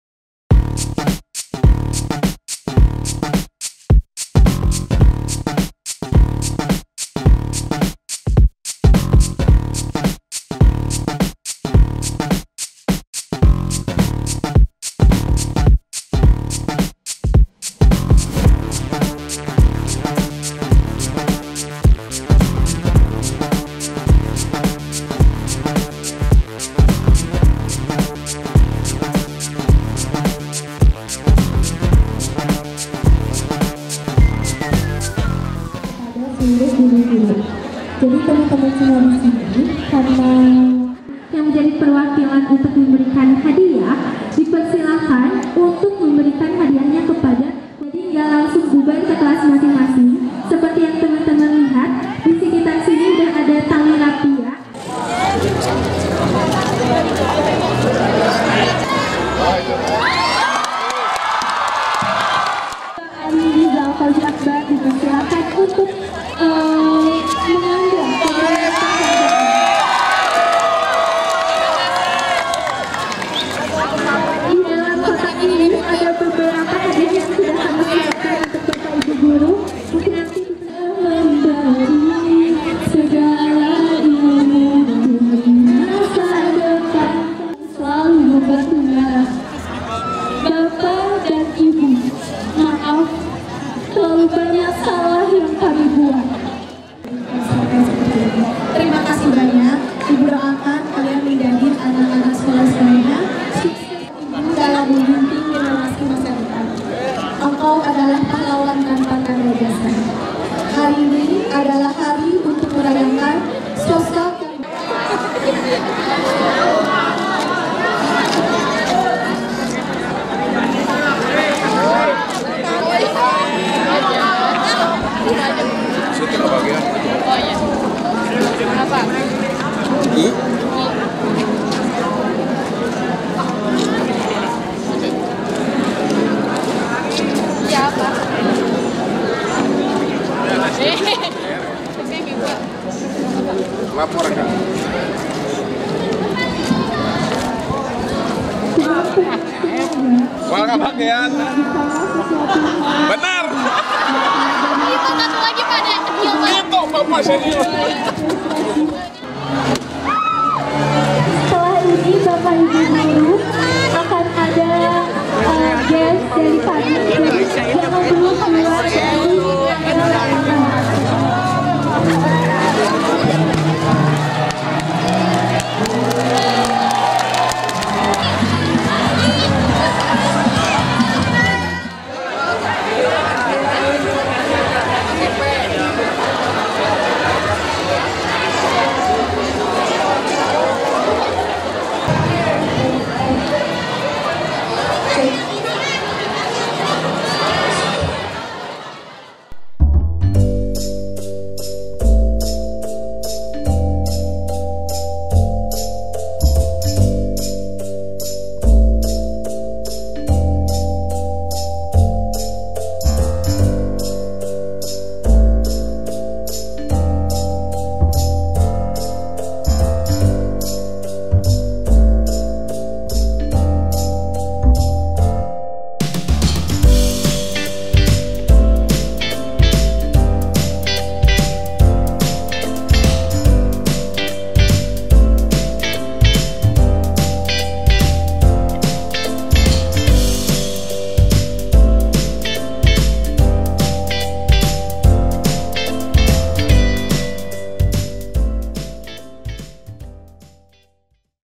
SELAMAT HARI GURU NASIONAL 25 NOPEMBER 2023
Kegiatan siswa-siswi SMA dan SMK Almarwah dalam memperingati Hari Guru Nasional pada Sabtu, 25 Nopember 2023.